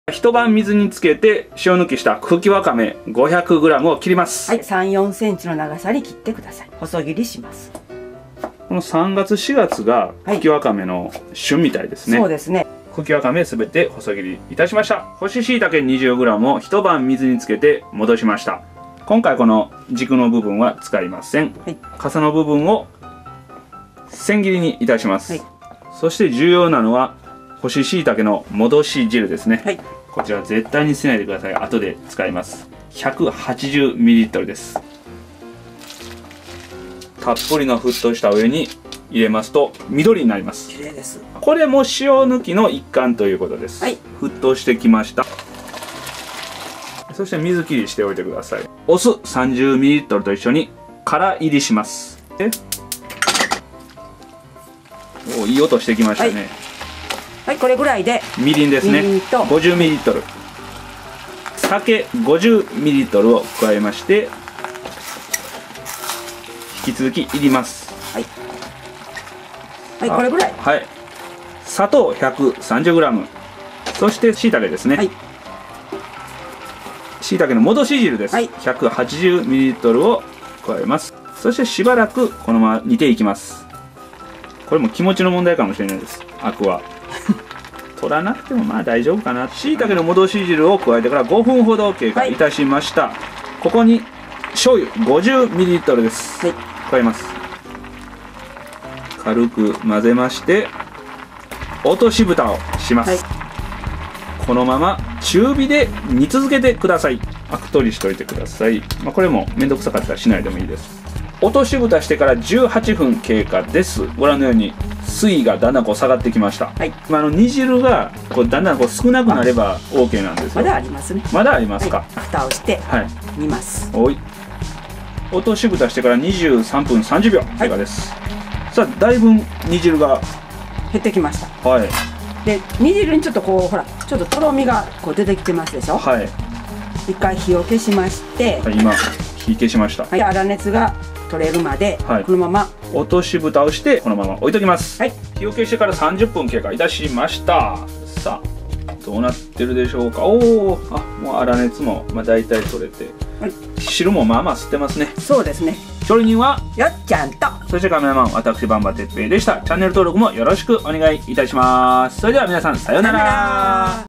茎わかめ佃煮レシピ
【材料】①茎わかめ500g②酢30ml③みりん酒しょう油各50ml④砂糖130g⑤干し椎茸20gと干し椎茸戻し汁180ml

0:00 ひと晩水に浸して塩抜きした茎わかめを細切りにする
0:16 ひと晩水に浸して戻した干ししいたけを切る
0:35 茎わかめを茹でる
0:49 酢とワカメを鍋で乾煎りする
0:58 各種調味料を加え乾煎り
1:12 更に調味料を加え煮る
1:34 しょう油を加え中火で煮る
2:17 消火してあら熱が取れれば完成
2:36 調理よっちゃん・カメラ哲平
2:42 ご視聴ありがとうございました